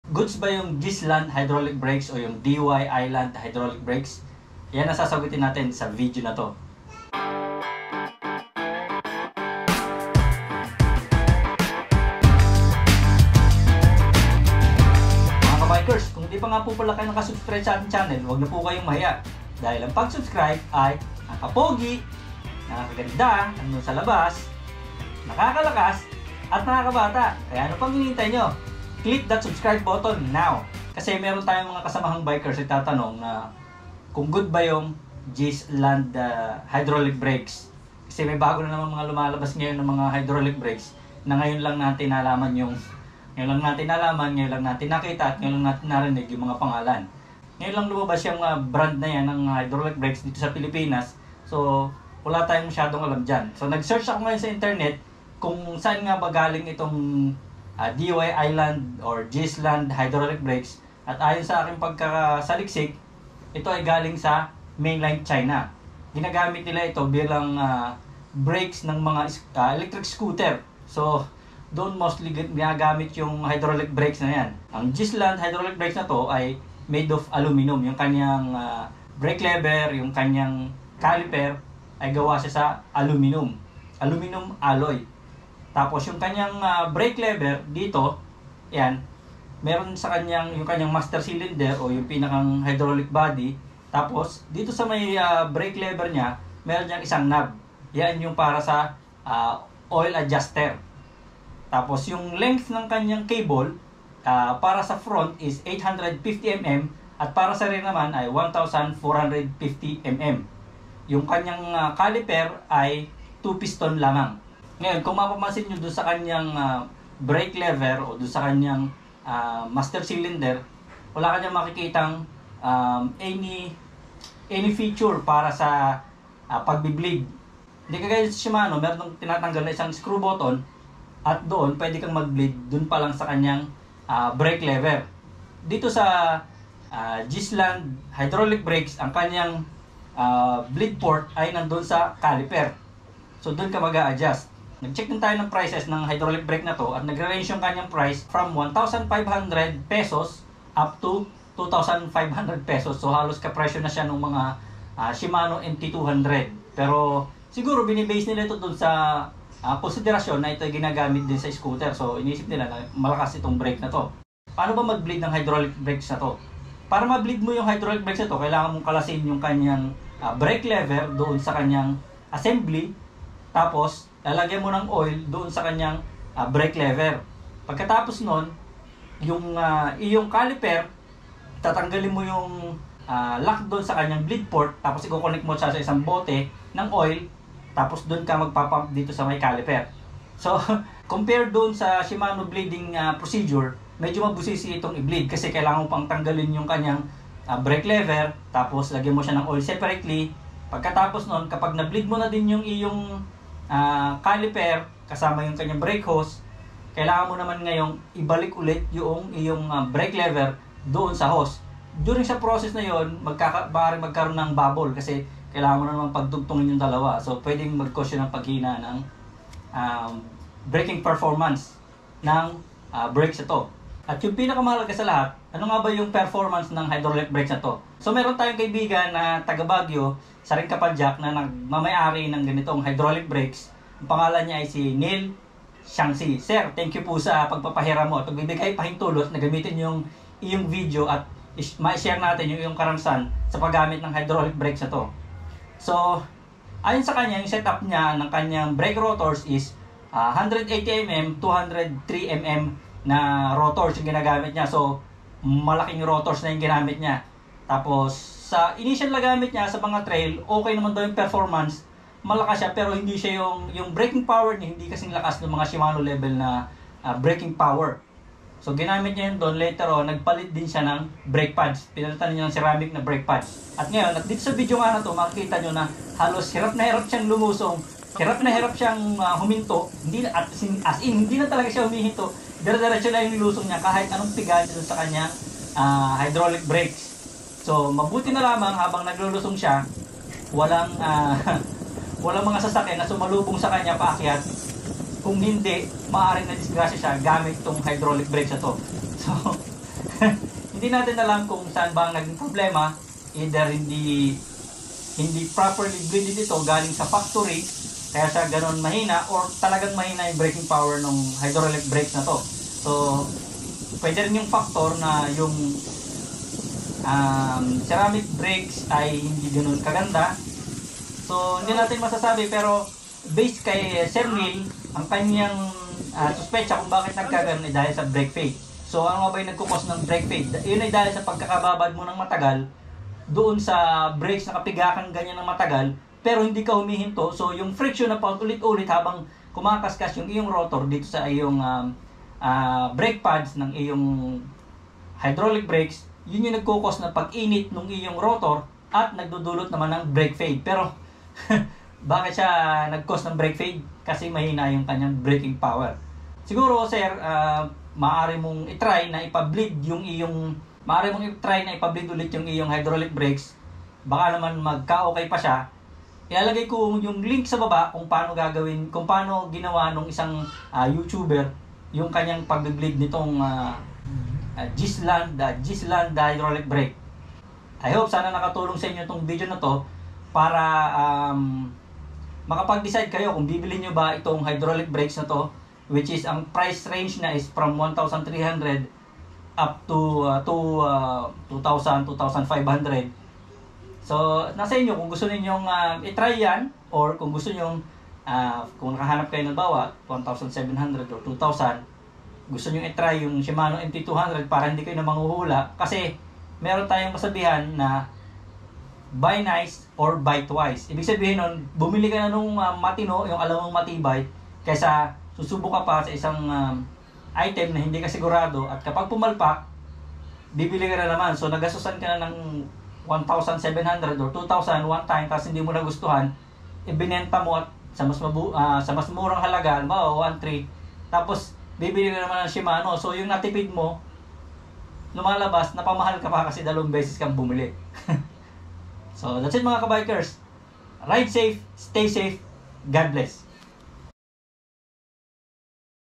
DYISLAND HYDRAULIC BRAKESET REVIEW - GOODS BA?
Sa video na eto ay ating irereview ang bagong lumalabas na hydraulic brakes Philippine market na Dyisland hydraulic brakeset. Isa etong budget brakes, pero depende sa seller, minsan halos na kapresyo na din eto ng Shimano MT200. Ginawa natin ang video na eto para magkaroon ng idea ang mga naghahanap ng budget brakes kung ok nga ba o hindi ang brakeset na eto.

Link on how to bleed this brakeset:
DYISLAND About the hydraulic brake how to change oil